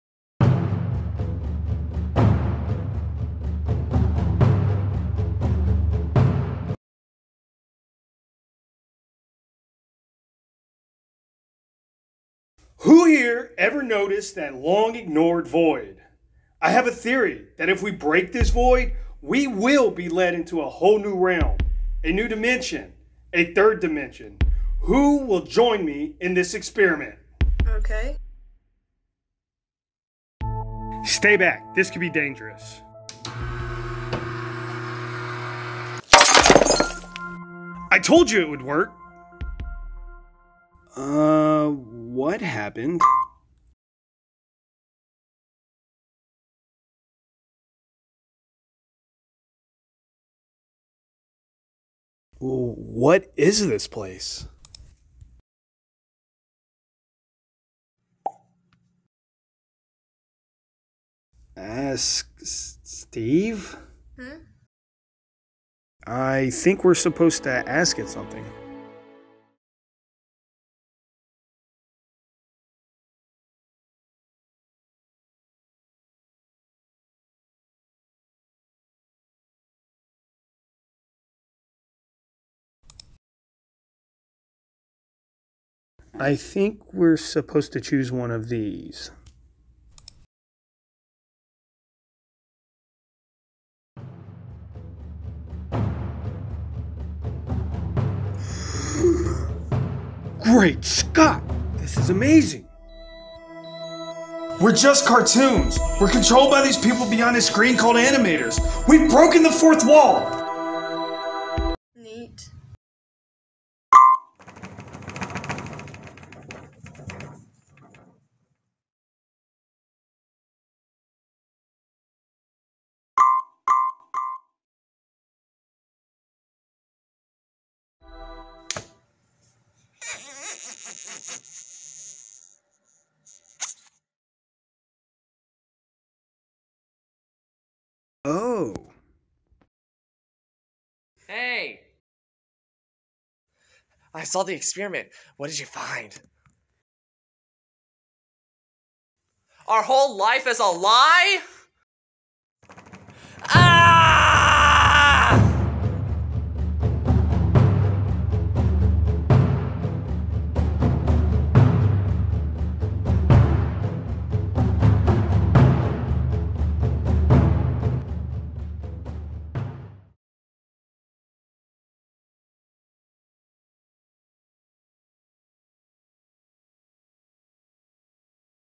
Into Reality
A UIL Film Animation creation by Jonah Johnston, Mason Cook, Maria Rios, & Zoe Floyd of Rusk High School. Submission is for the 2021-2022 school year.